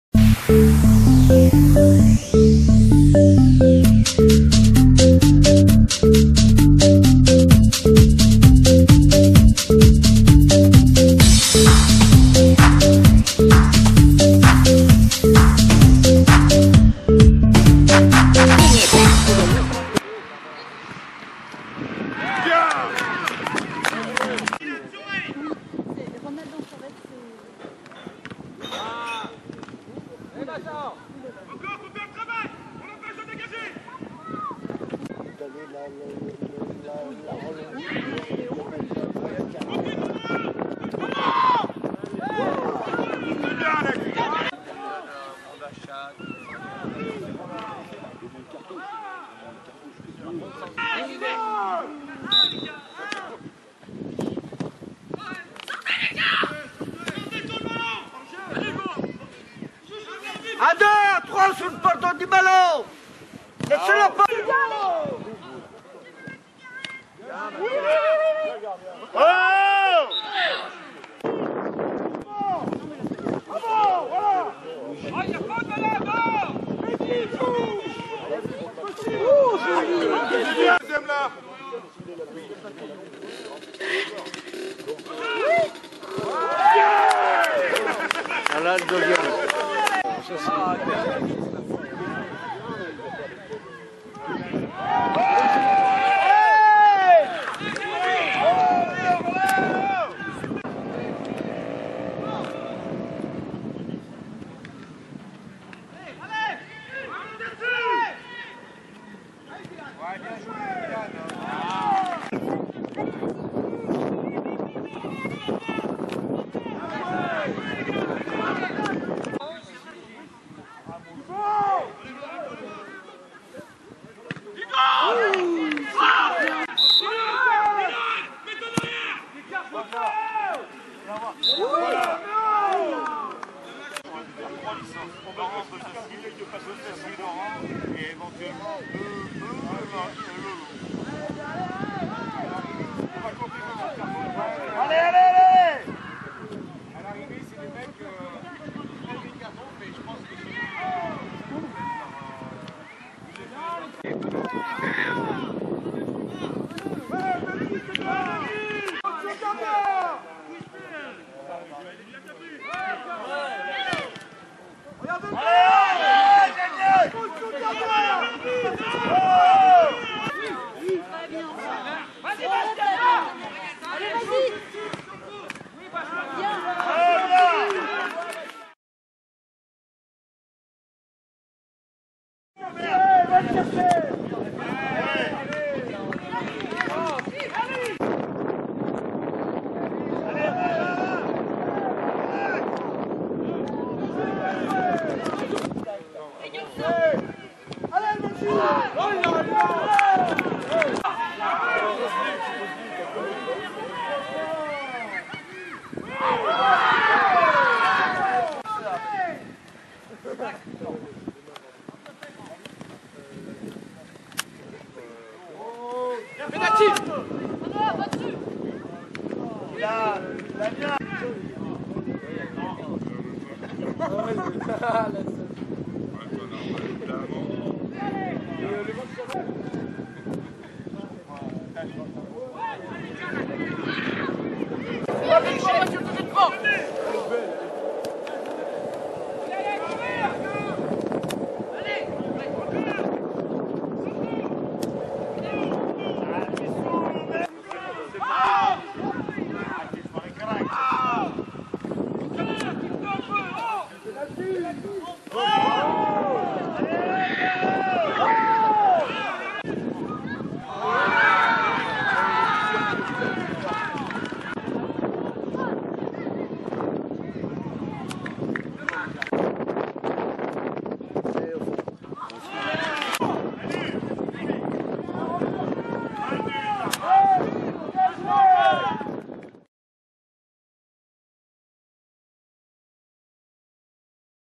2012 2013 U15 Chandieu vs Montchat
FCO Chandieu vs Montchat 2
Match de football du district du Rhône, saison 2012/2013
Catégorie U15 - 15/12/2012